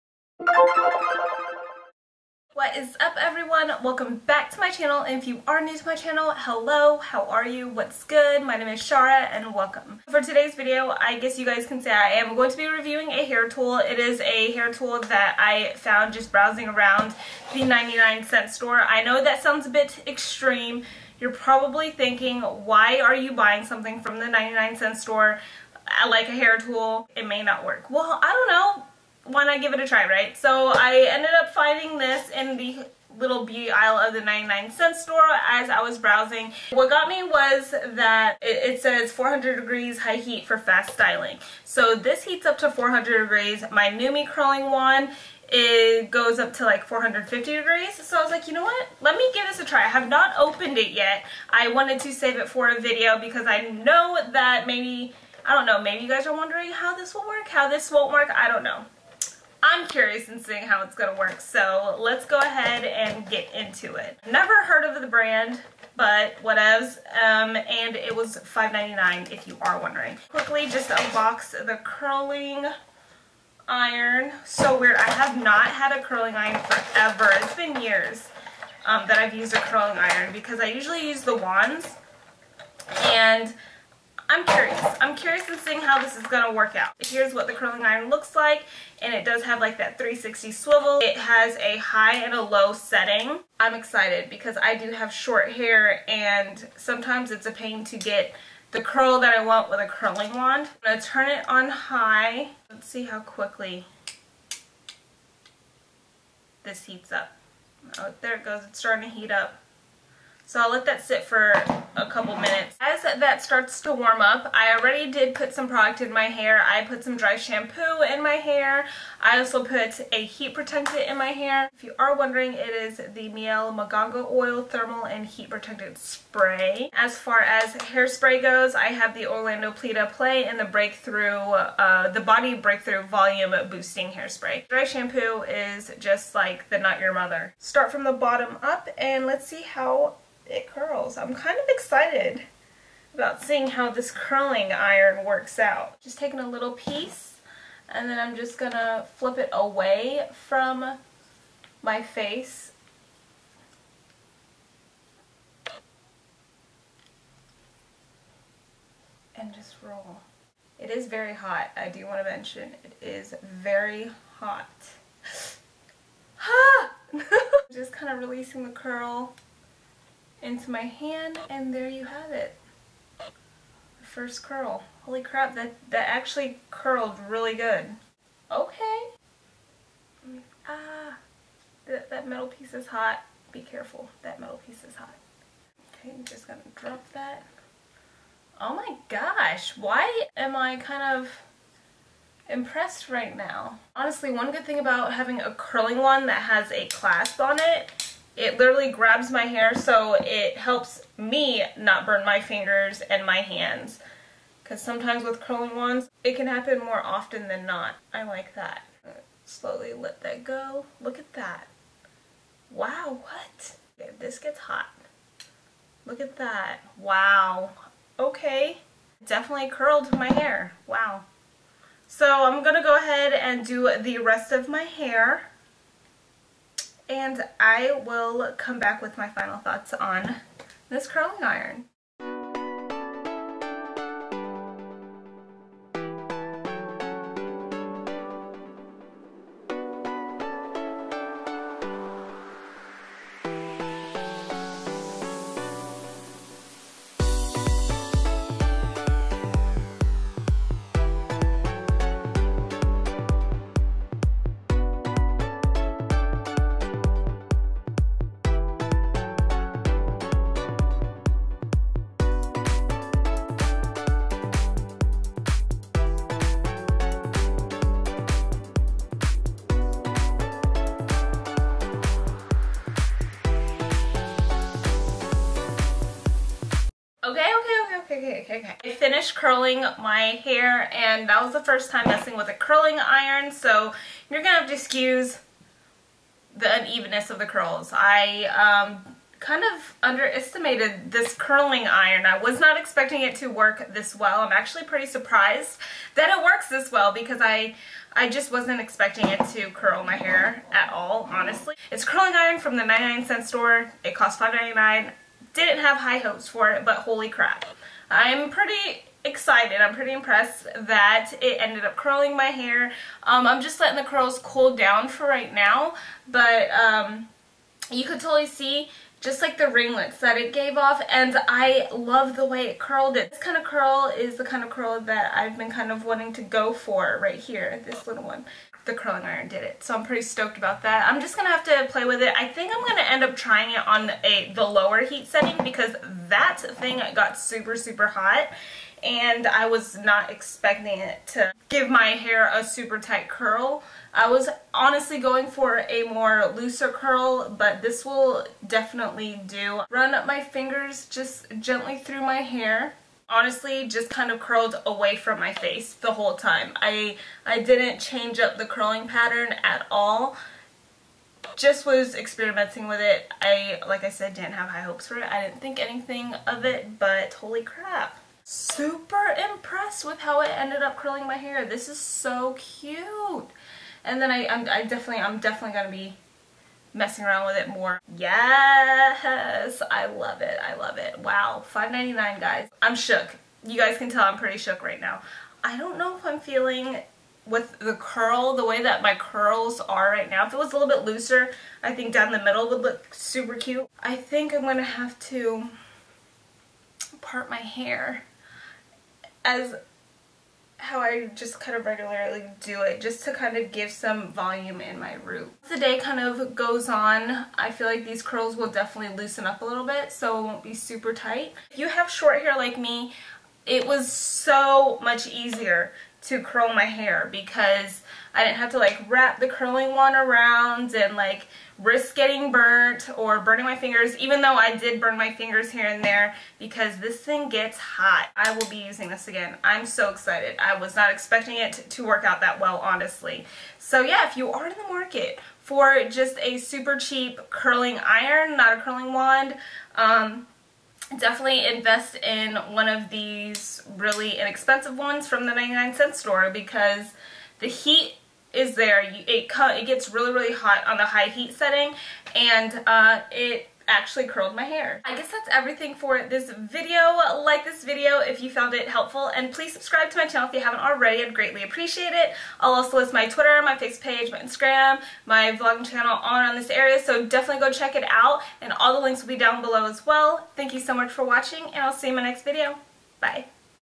$5.99 Curling Iron at the 99 Cent Store | REVIEW!
Helloo gorggiess!! I came across this curling wand at the 99 cent store and was skeptical about it. So I gave it a go and filmed a video on how well it worked. I was pretty shook at the results! Enjoy!

Lets keep in touch! :D
__SVC0306 (those are TWO underscores!) :)
Personal Instagram: shaira0717